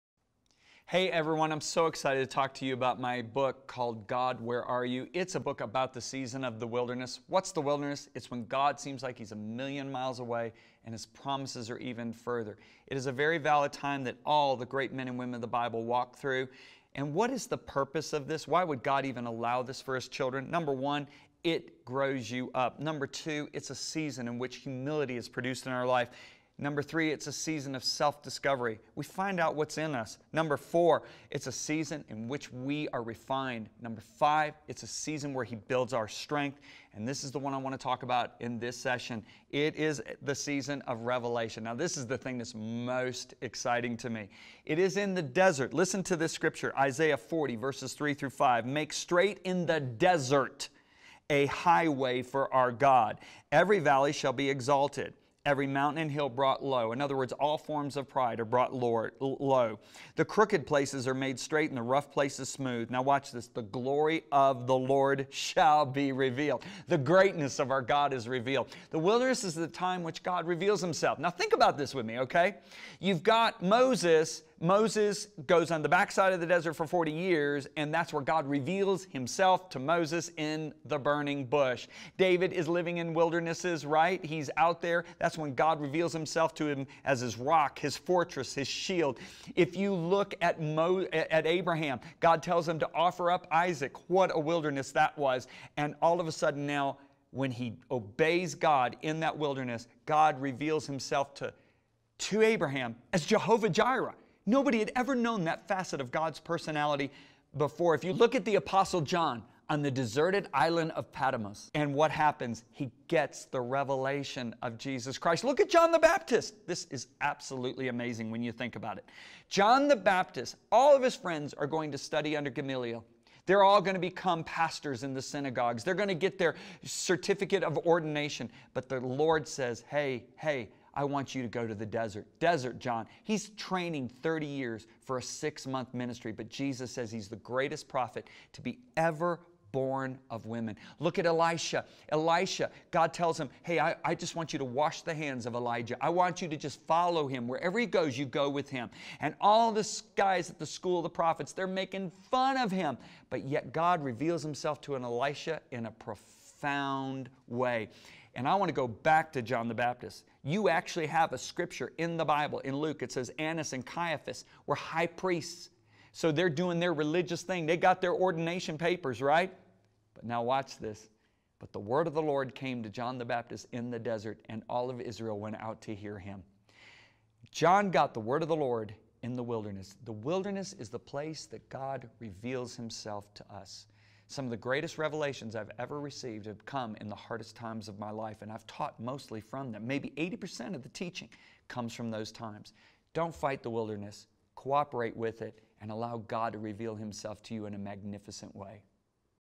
God Where Are You? / JOHN BEVERE - GOD REVEALS HIMSELF
John Bevere is best selling author of the new book GOD WHERE ARE YOU? He joins us all this month  with teachings and interviews with Cheryl Weber.